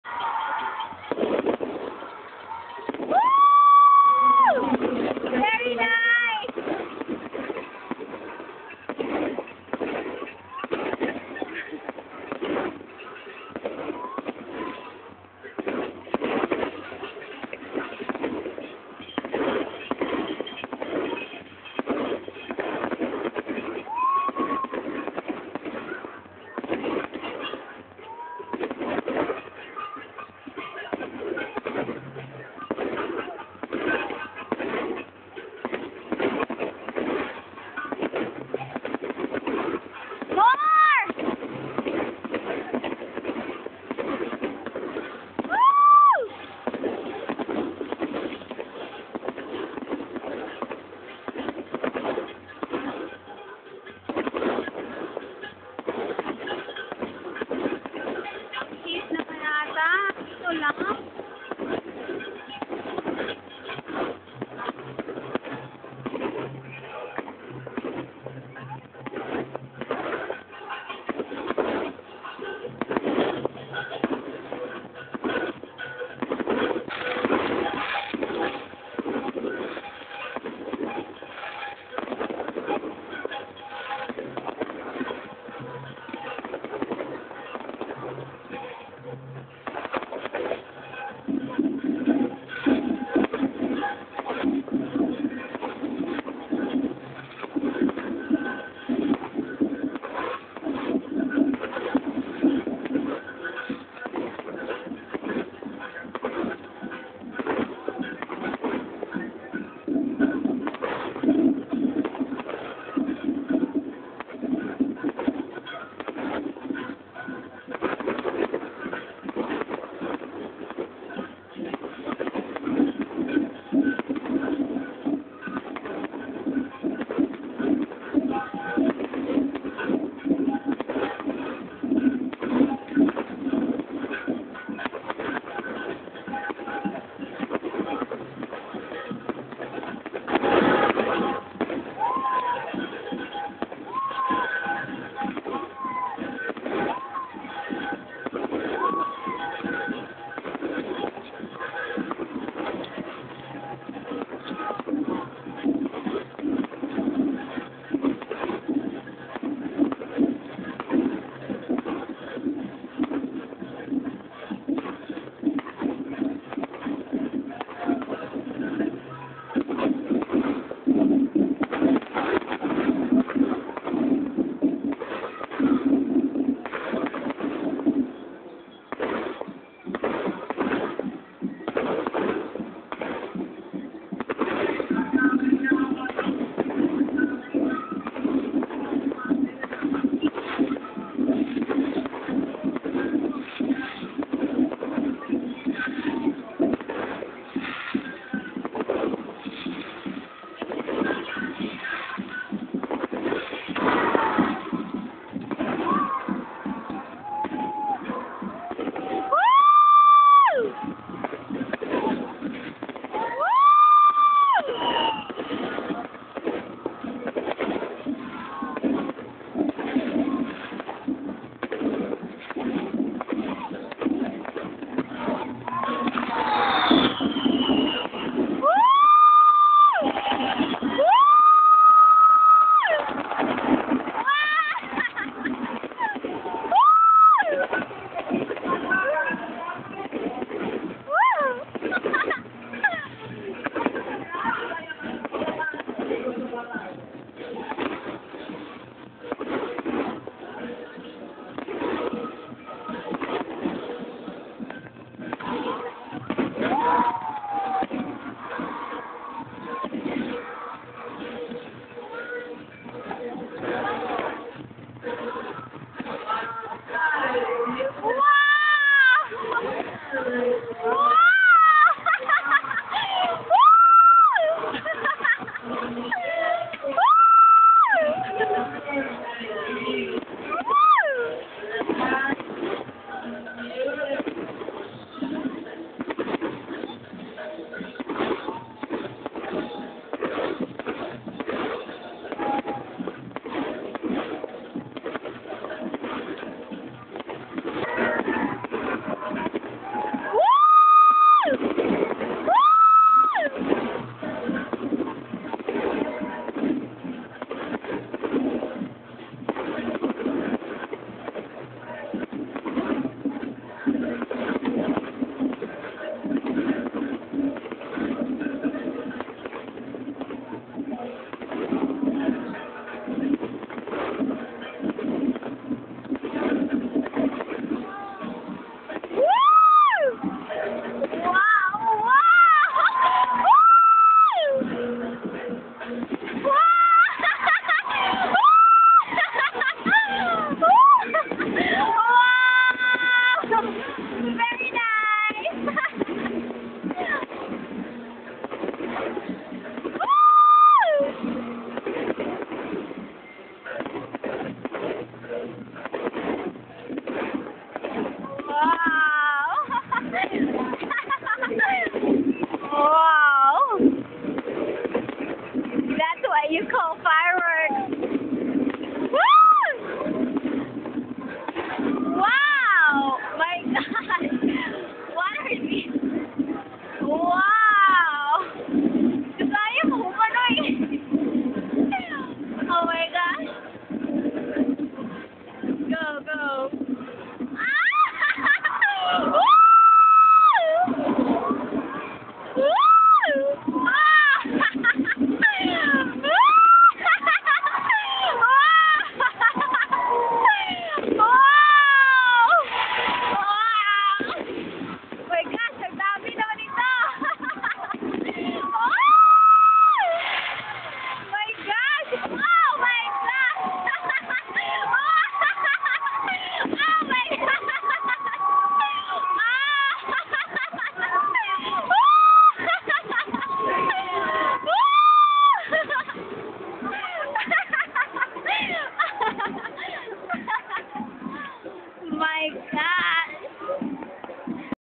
Sandugo Fireworks 2009
fireworks display during sandugo festival last july 27 2009 held at the grandstand, bohol